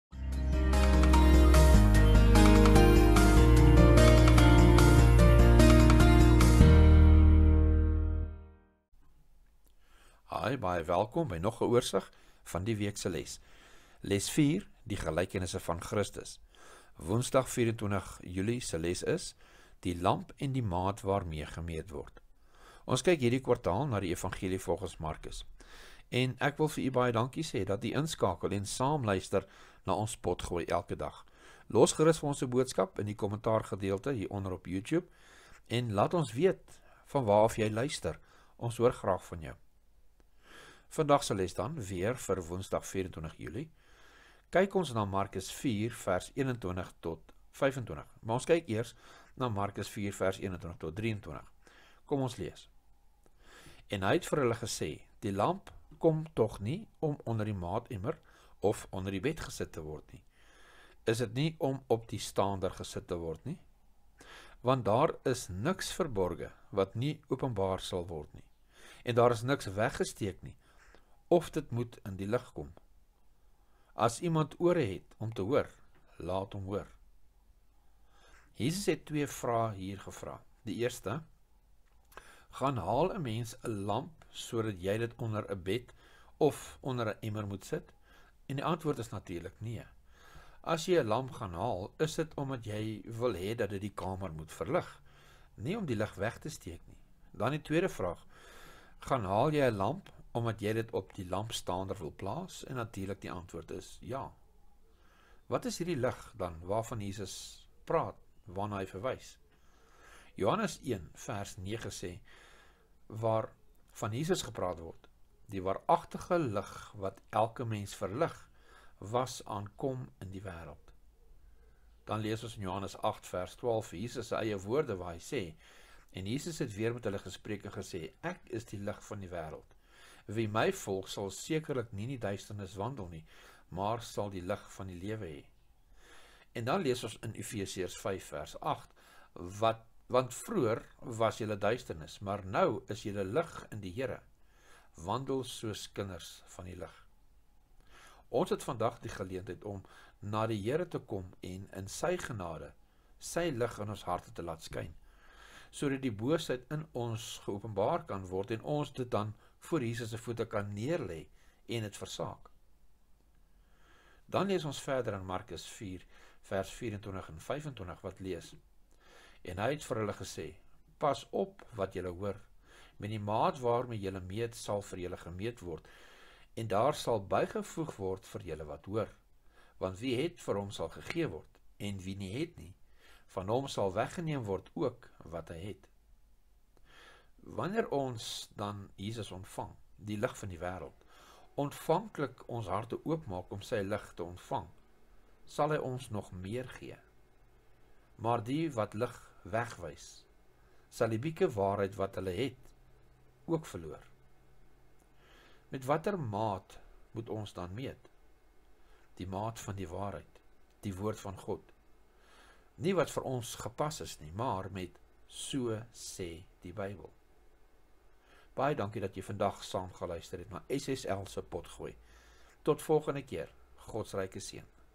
Woensdag 24 Julie - Die lamp en die maat waarmee gemeet word.
Les 4 –  Die gelykenisse van Christus
20 tot 26 Julie

Derde Kwartaal 2024 – DIE EVANGELIE VOLGENS MARKUS

Aanbieder:
David Kühn

CREDITS:
CREDITS: Alle regte is die van die Wereld Konferensie van die Sewende-dag Adventiste Kerk.
Jesus Christus.